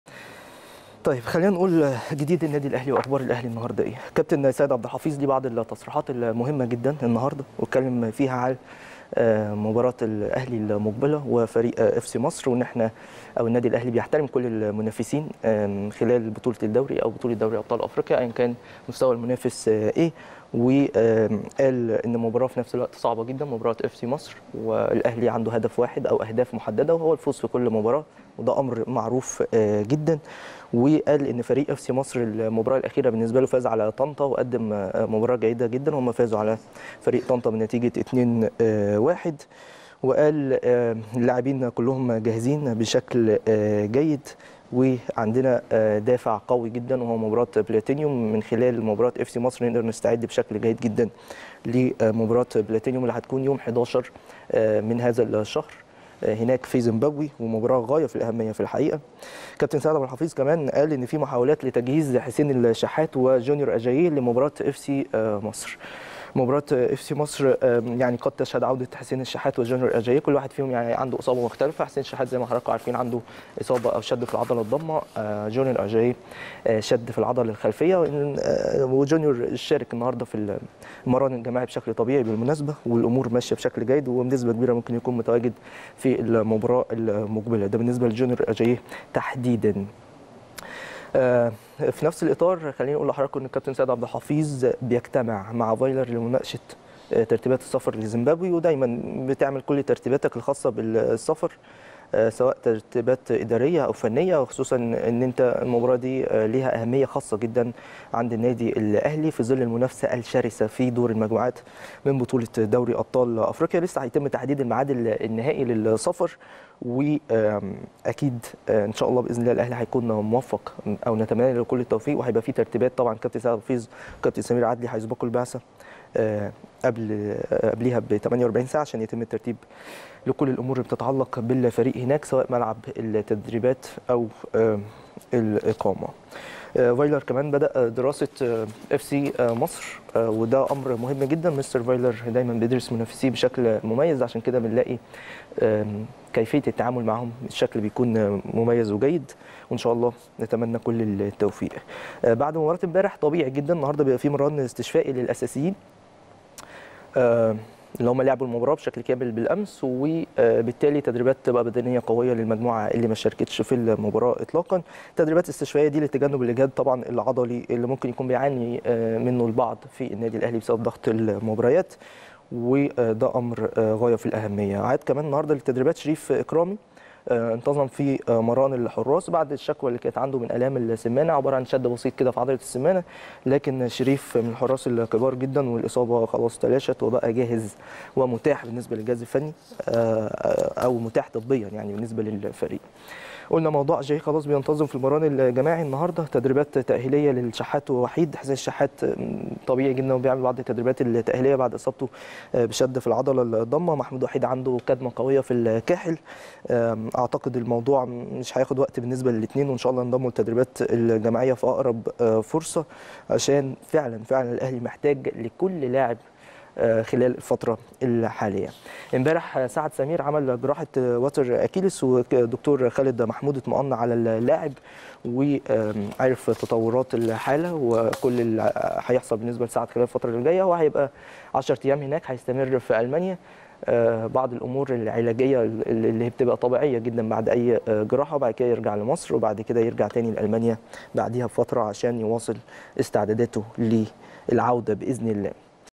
امير هشام: عبد الحفيظ يجتمع مع فايلر لمناقشة ترتيبات السفر لزيمبابوي
التردد: 12341 أفقي.
القمر: نايل سات.
معامل الخطأ: 5/6.
معدل الترميز: 27500